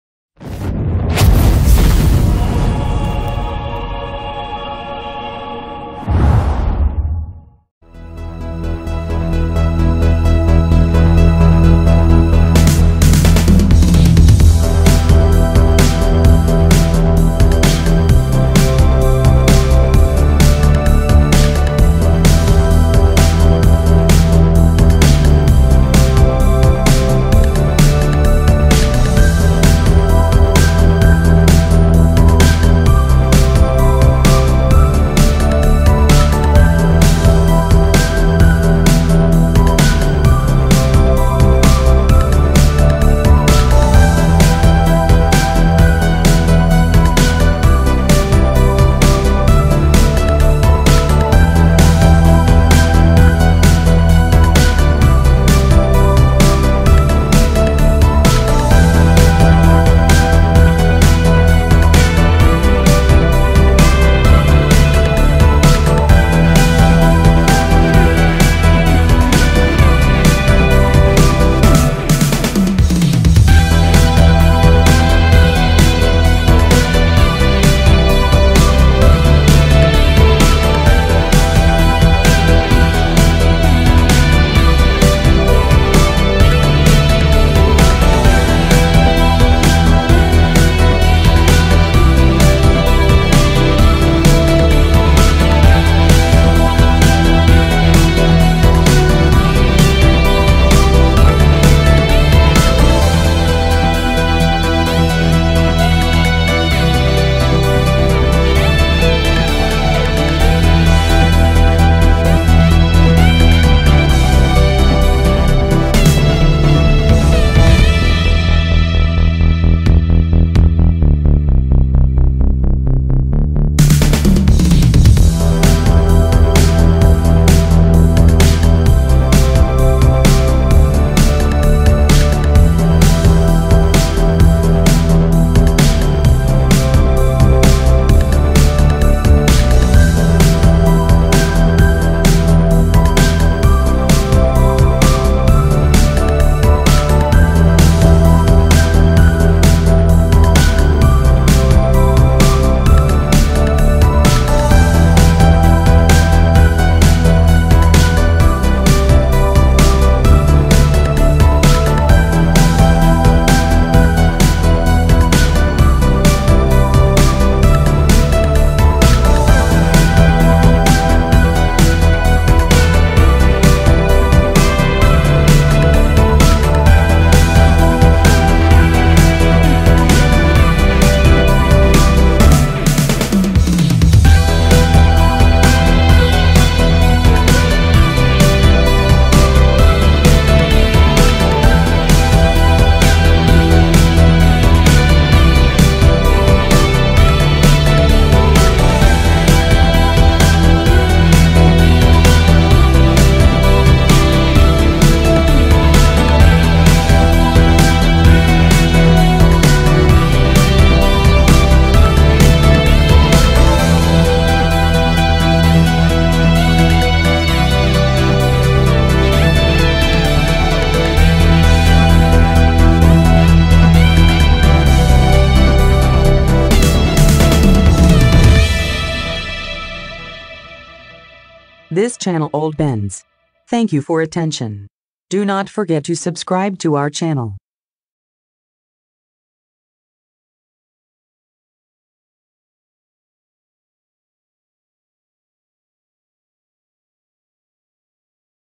1986 Mercedes-Benz 560 SEC 6.0 AMG Wide-Body - the white dream
What could be better than the Mercedes-Benz 560 SEC c126? Only Mercedes-Benz 560 SEC 6.0 AMG Wide-Body. 6.0-liter V-8 engine, automatic transmission. Unique 'wide-body' conversion with numerous AMG features. Interior features an AMG Nardi steering wheel and Recaro sports seats.